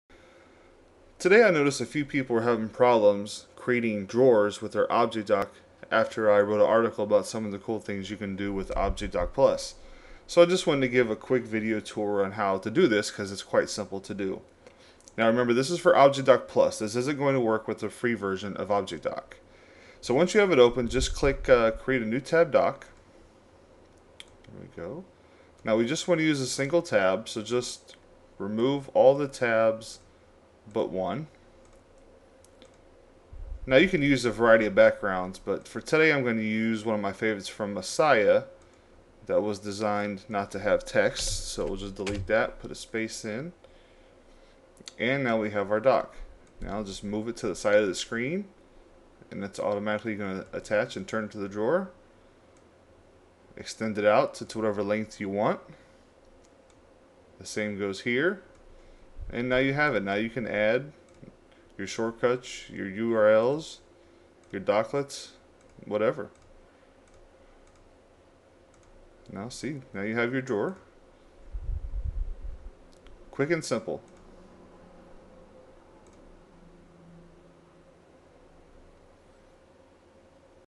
Creating an ObjectDock Drawer
This video shows how to configure ObjectDock as a drawer on your desktop.